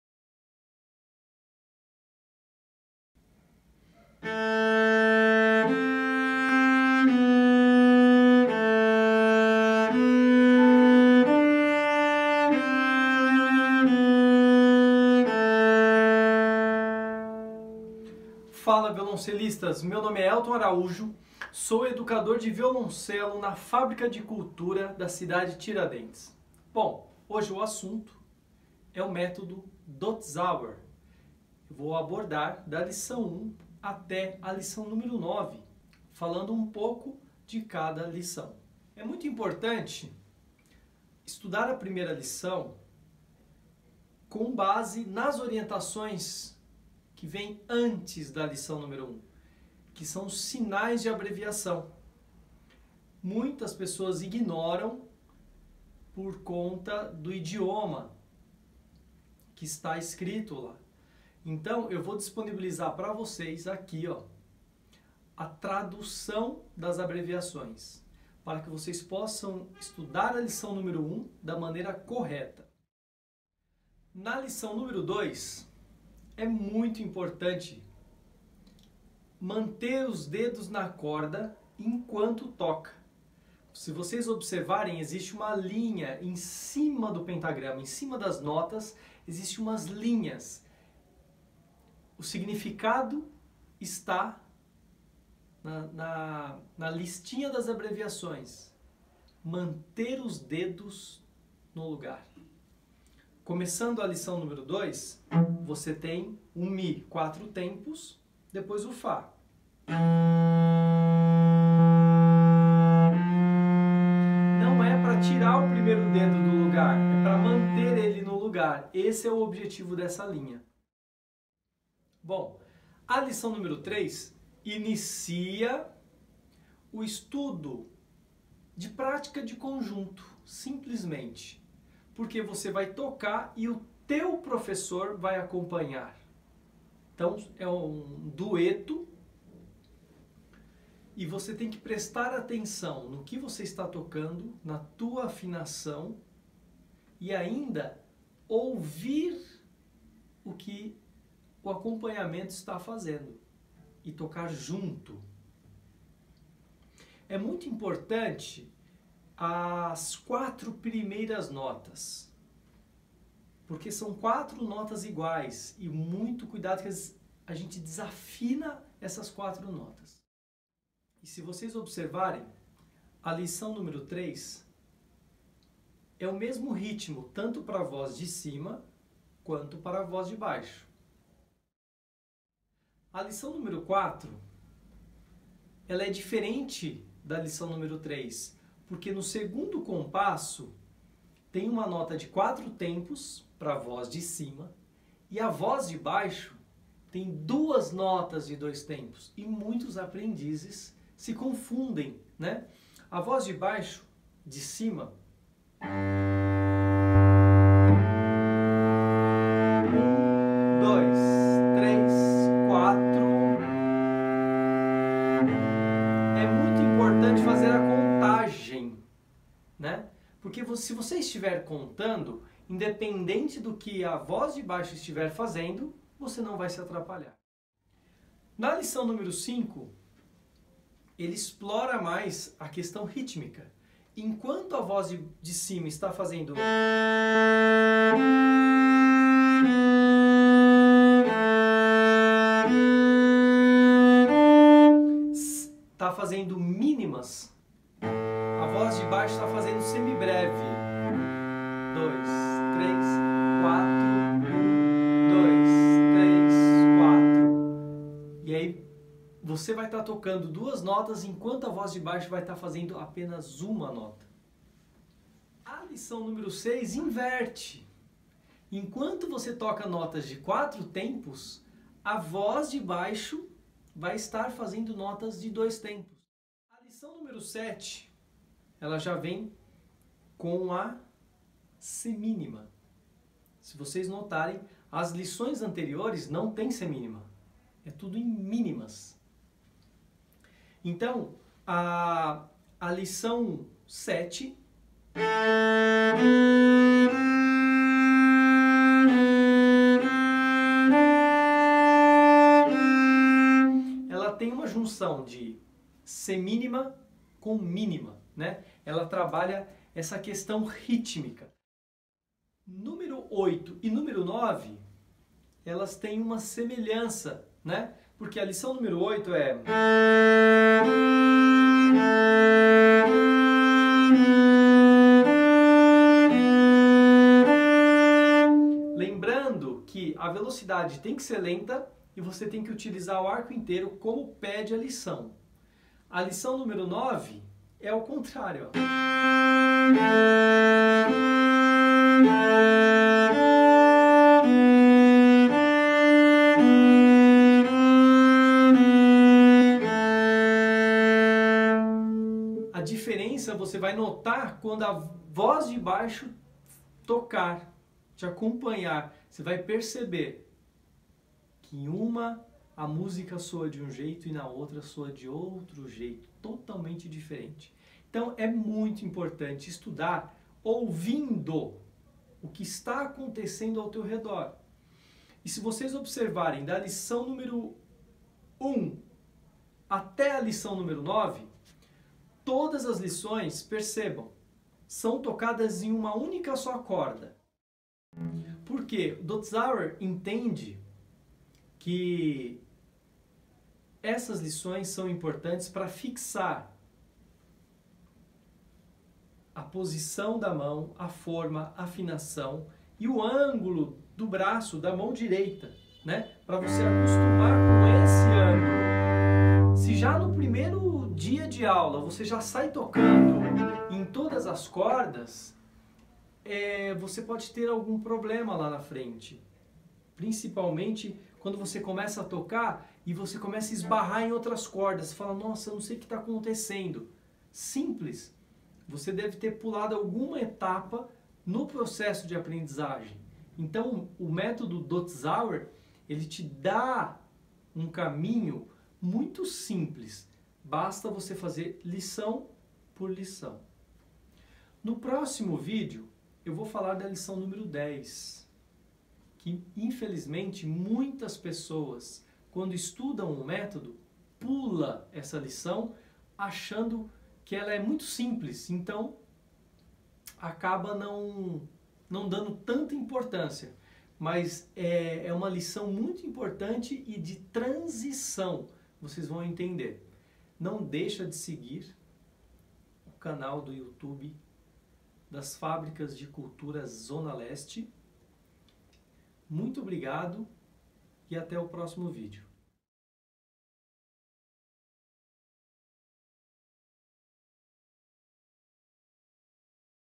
Aula: ''Violoncelo Orientação e introdução ao método Dotzauer lição 1 a 9 '' (FC Cidade Tiradentes)
O educador Elton Araújo aborda postura, posicionamento do instrumento, empunhadura do arco e produção sonora. Orientação a cada lição, como preparar o estudo.